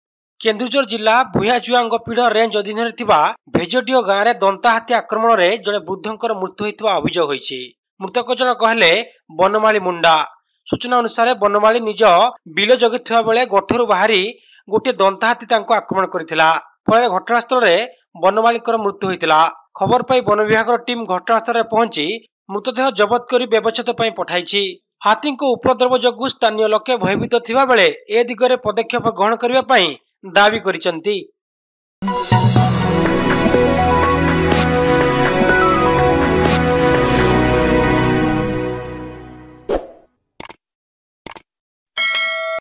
ଦନ୍ତା ହାତୀ ଆକ୍ରମଣରେ ବୃଦ୍ଧଙ୍କ ମୃତ୍ଯୁ, କେନ୍ଦୁଝର
In a tragic incident in the Bhuyan Juang division of Keonjhar district, Banamali Munda lost his life in an attack by a tusker elephant in Vejdiha village. The forest department swiftly responded, retrieving the body for post-mortem. The local community expresses fear due to elephant encounters, urging authorities to take necessary actions for future safety.